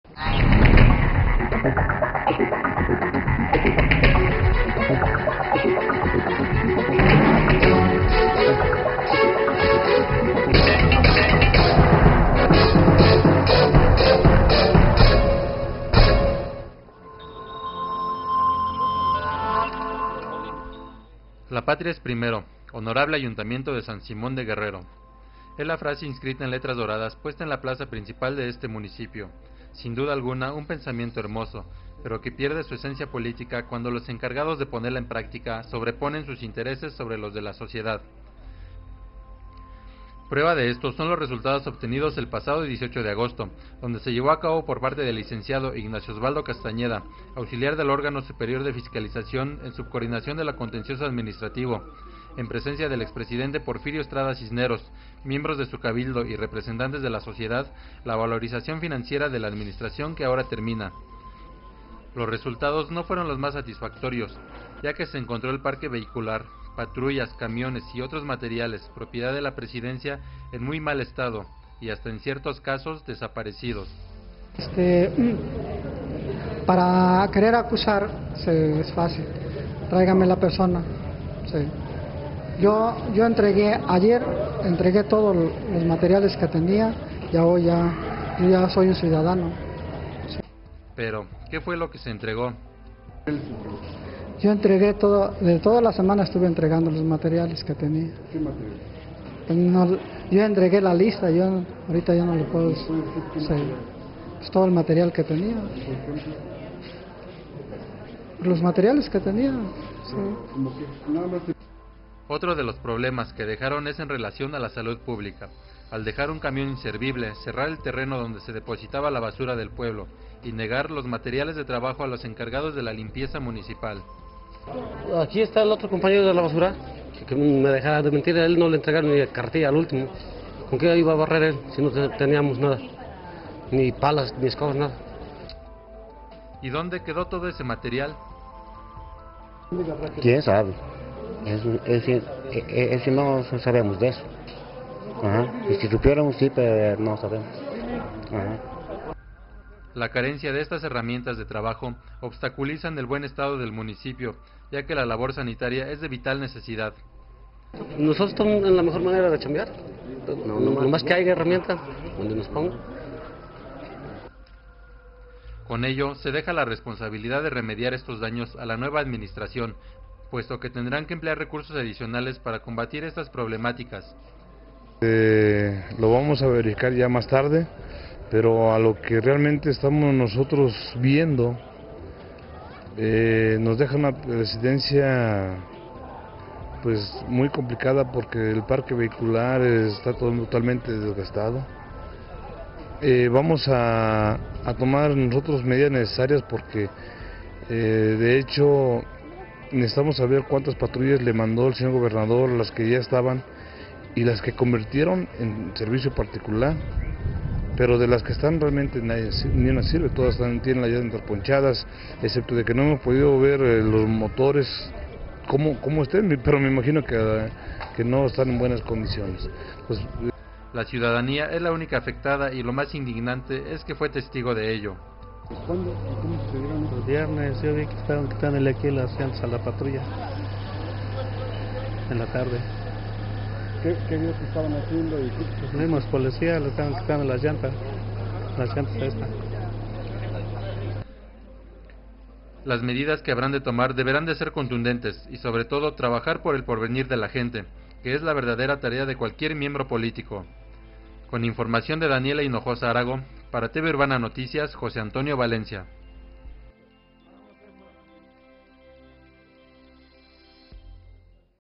Entregan administracion municipal con anomalías: San Simón de Guerrero
Porfirio Estrada Cisneros entrega administración con anomalías